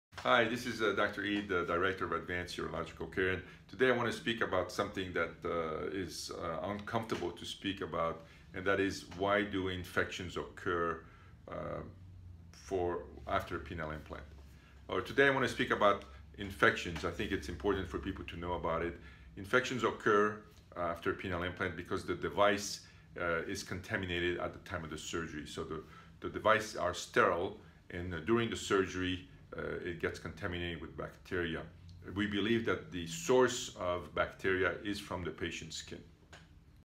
Q/A Series: Why do infections occur?
Dr. Eid discusses his thoughts on why infections occur with penile implant surgery.